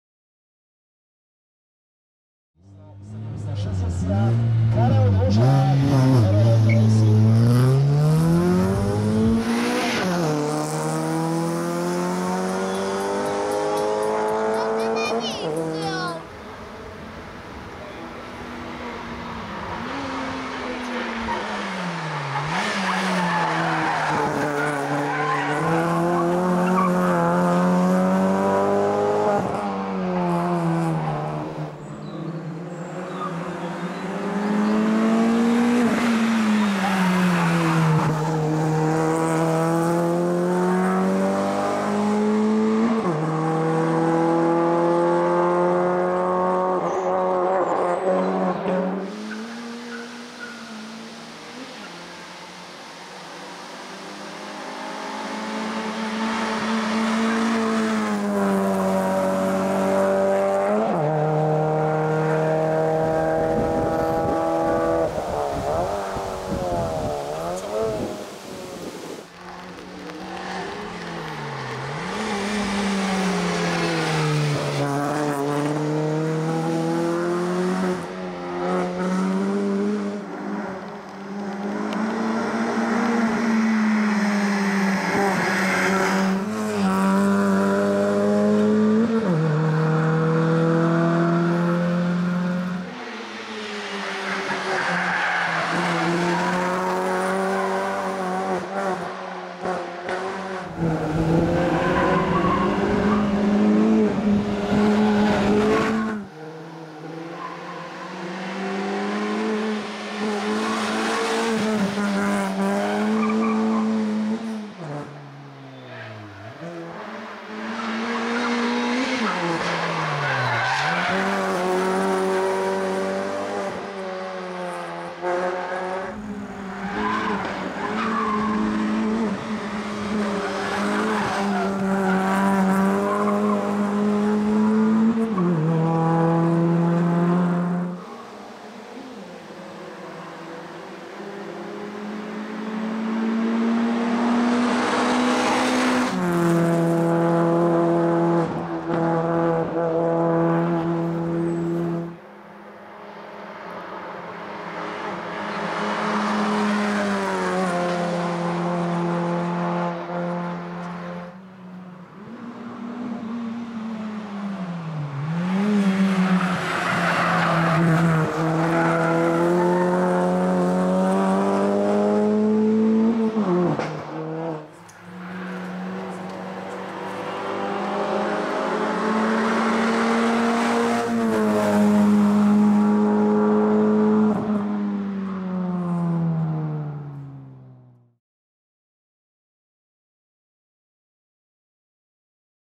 Karel Dvořák - Citroën C2 VTS - BABA 2020 - Pezinok - EDDA CUP
Karel Dvořák si s vozem Citroën C2 VTS dojel v sobotu pro 3.místo v Saxo Cupu v závodech do vrchu BABA 2020 u Pezinku.